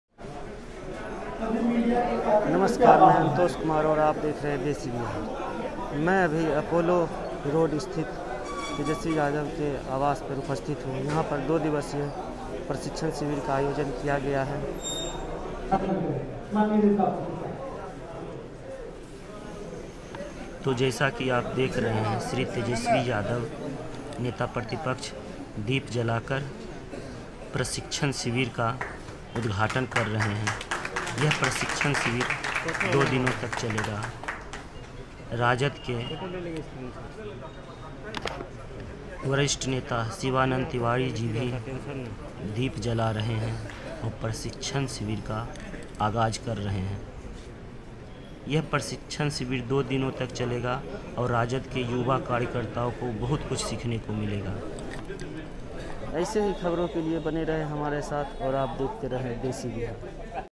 दीप जलाकर दो दिवसीय प्रशिक्षण शिविर का उद्घाटन करते नेता प्रतिपक्ष श्री तेजस्वी यादव...
'देसी बिहार' न्यूज़ चैनल में आपका स्वागत है।  बिहार से जुड़ी हर छोटी बड़ी खबरों के लिए बने रहे हमारे साथ।